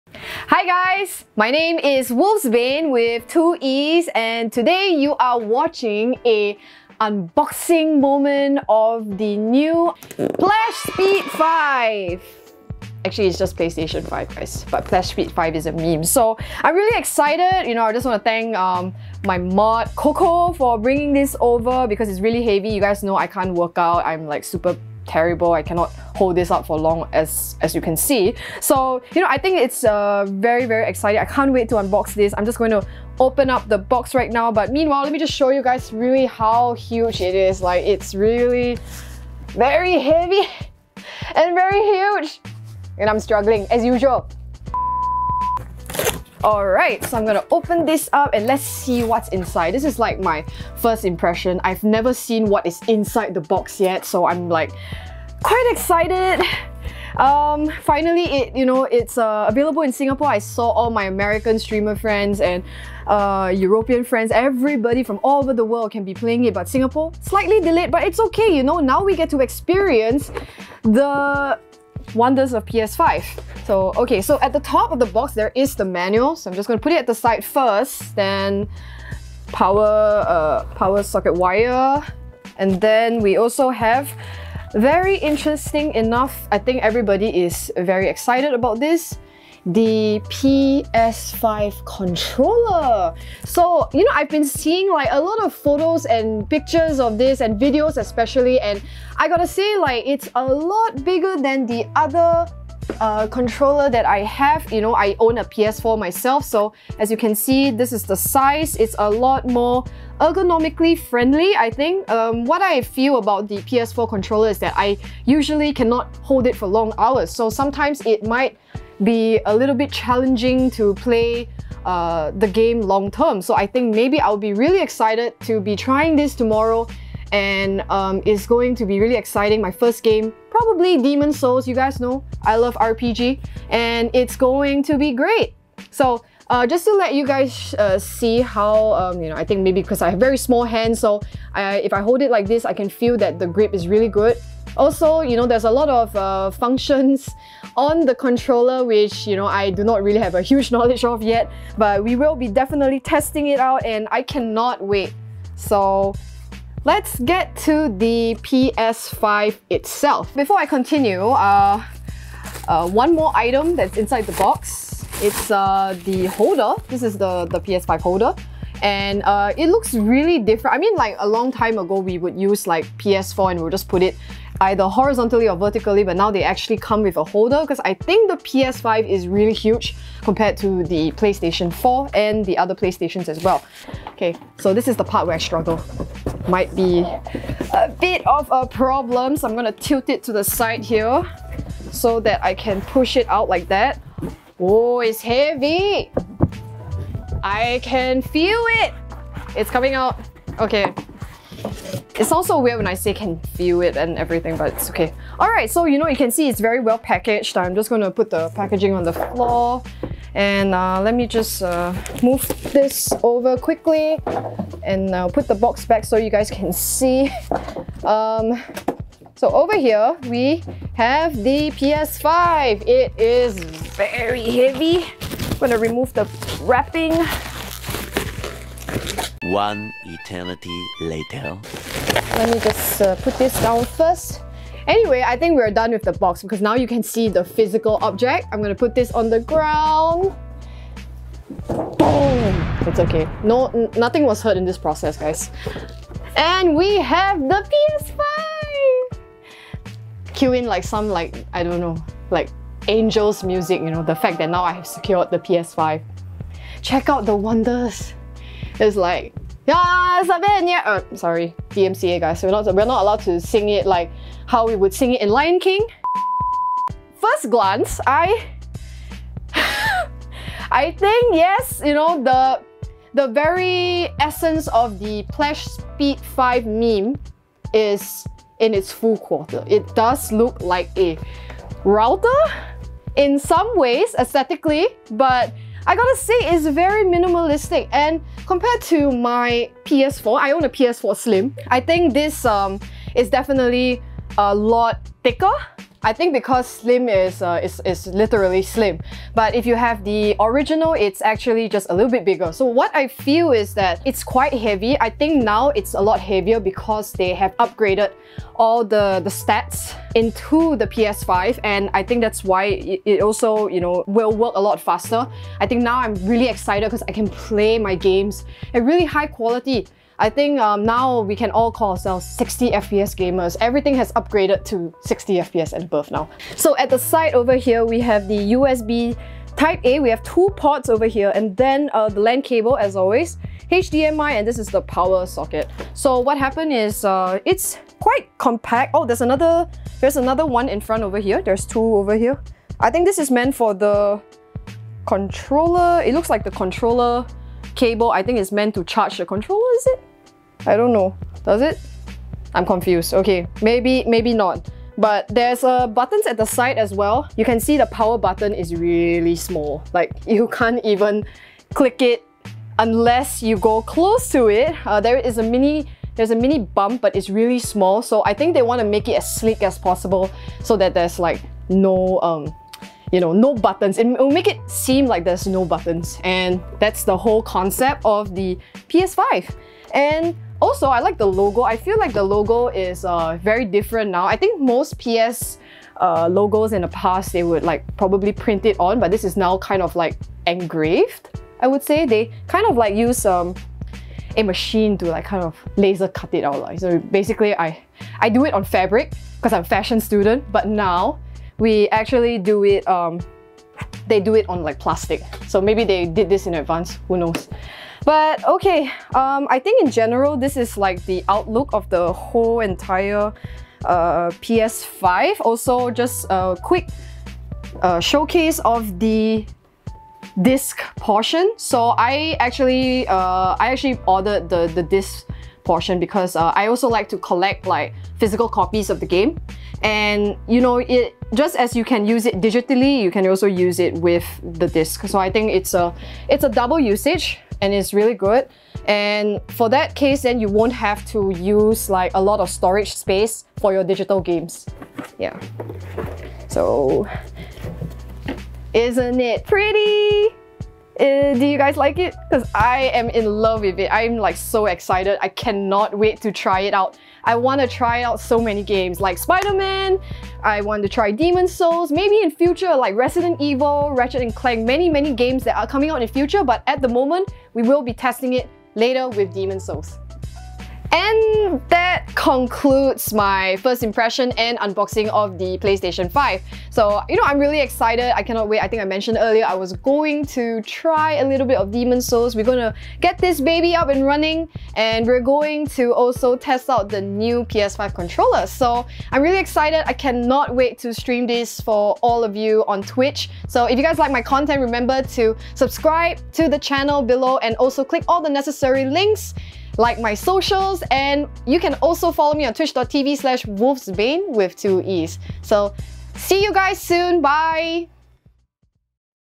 I UNBOXED MY FIRST PS5! - FIRST IMPRESSIONS
I am now a proud owner of the Playstation 5 ! Watch how I unbox the PS5 and also my first impressions on it.